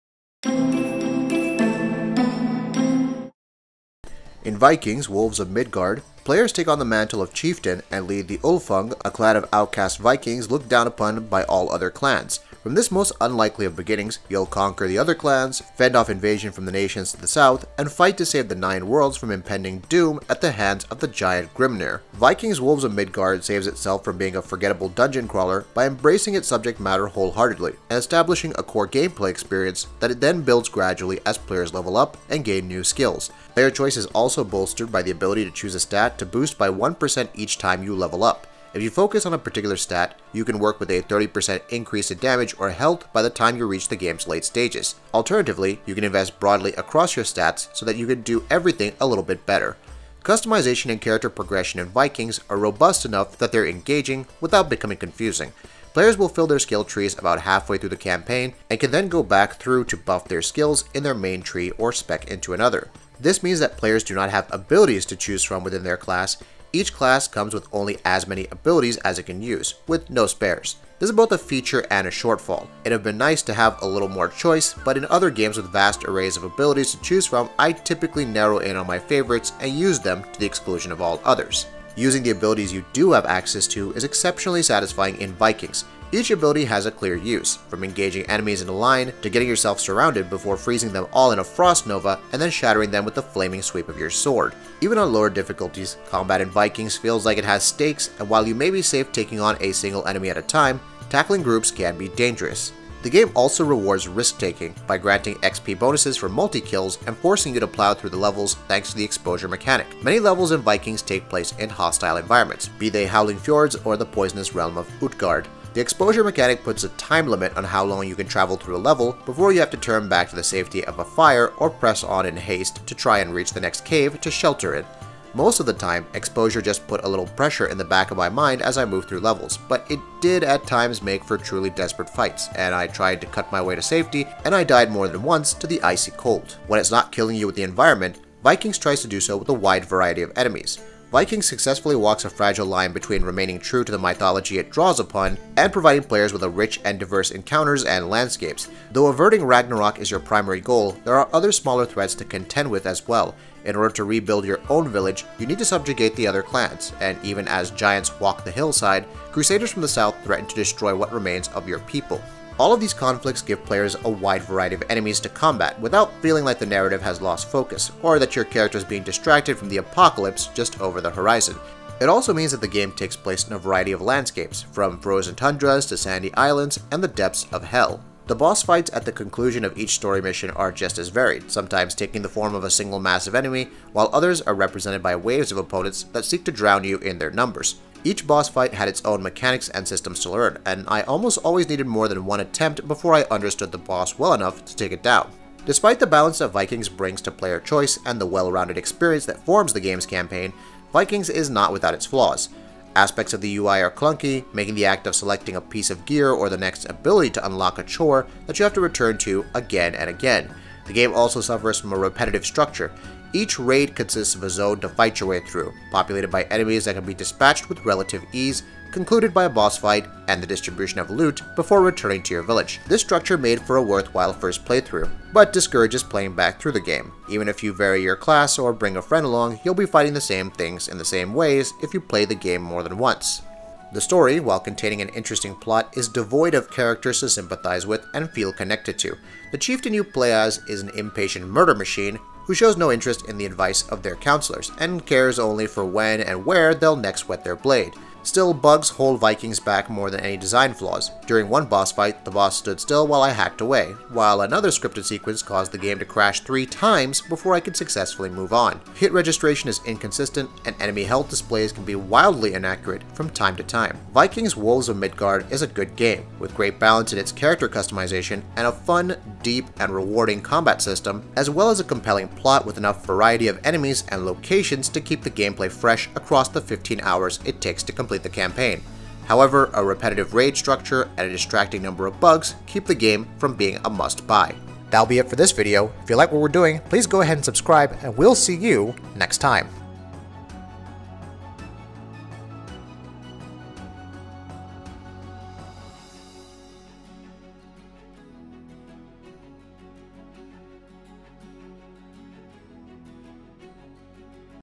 Vikings: Wolves of Midgard Review - The Final Verdict
Vikings: Wolves of Midgard is a recommendable dungeon crawler, with fun and interesting enemies, a wide but not overwhelming array of player abilities, and an engaging combat system that demands that players remain on their toes and think carefully about what abilities to use and when. However, a few technical issues, and a repetitive structure prevent the game from being something for players to return to after they’ve completed the main story.